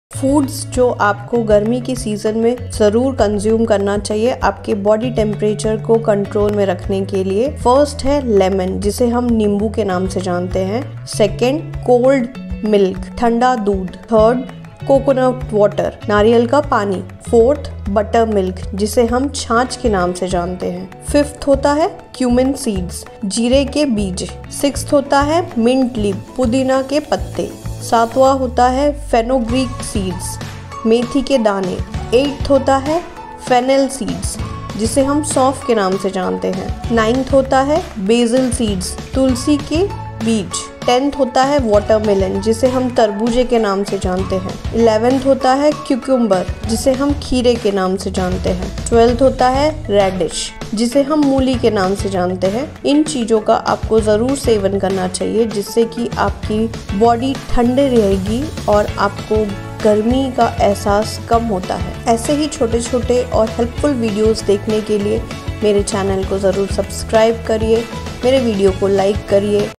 Beat the Heat | Summer Cool Food Items | 12 गर्मियों में ठंडी खाने की चीज़ें | Reduce Body heat
हीट बीट फूड्स लिस्ट | Beat the heat with these 12 foods | Foods that help reduce body heat

Hi, guys in these video I have shared 12 foods to beat the heat.

LEMON: Lemon helps in lowering body heat. A glass of lemon water in the morning with empty stomach helps you to keep your body cool. Luke warm water with lemon juice and honey will help to reduce excess fat.

COLD MILK: Cold milk help to keep body cool by reducing body heat. Have a glass of cold milk by mixing almond powder/soaked almond puree during breakfast or in the night. Cold milk also helps to reduce acidity.

COCONUT WATER: It keeps body cool and hydrated due to high water content and helps to overcome the problems such as dehydration and summer infection. Coconut water is rich source of Vitamin C and minerals (Calcium, Magnesium, Phosphorous, Zinc & Potassium). Coconut water provide relief if you are facing digestion problem.

BUTTERMILK: Buttermilk helps to keep the body cool and hydrated. It is suggested to drink at least a glass of buttermilk everyday and do not avoid specially during summer season. Addition to Ginger, Cumin Powder, Coriander, Mint will help to improve your digestion.

CUMIN SEED/JEERA: Cumin Seeds also have cooling properties. Soak cumin seed in the water and drink it in the morning. You can also eat cumin powder by mixing it with buttermilk.

MINT/PUDINA: You can chew few mint leaves daily to overcome heat. If you don't like chewing, it can be consumed by adding it to buttermilk, chutney or with some fresh fruit juices. Mint will enhance the flavor of buttermilk/fresh fruit juices.

METHI/FENUGREEK SEEDS: Methi/Fenugreek seeds are easily available ingredient which helps to reduce heat and keep the body cool. Methi/Fenugreek seeds are generally used while cooking Indian dishes. Drink Methi seed water – soak Methi seeds overnight, strain seeds in the morning and drink the water.

FENNEL SEEDS/SAUNF: To lower the body temperature, fennel seeds can also be used. Soak overnight, strain the seeds and the water in the morning. In Indian culture Fennel seeds are served after lunch/dinner.

BASIL/SABJA/TULSI SEEDS:  Basil seeds are known as Tulsi/Sabja Seeds. It helps to keep body cool by lowering body temperature. Soak it in water overnight & drink the water in the morning. No need to strain the seeds.

WATER MELON: Watermelon - name itself suggest that it is a high water - rich fruit and very effective in lowering body heat. Its keeps you hydrated and cool throughout the day. Enjoy few pieces of watermelon or you can prepare juice out of it.

CUCUMBER/KHIRA: Cucumber also has cooling benefits. Cucumber is very effective in reducing body heat. Eat cucumber anytime throughout the day and stay hydrated. You can have Cucumber Raita in lunch/dinner.

RADISH/MOOLI: Radish/Mooli is also one of high rich water content food. It can also be consumed to regulate body heat. Have it with regular meal in salads and cherish the benefits.

 Stress, dehydration and hormonal imbalance cause the body temperature to rise. The heat leads to indigestion, fatigue, burning sensation in the eyes and sometimes even a nose bleed. Here are a few foods that aid in cooling the body down.

Yours Truly
Shreya Chandrakar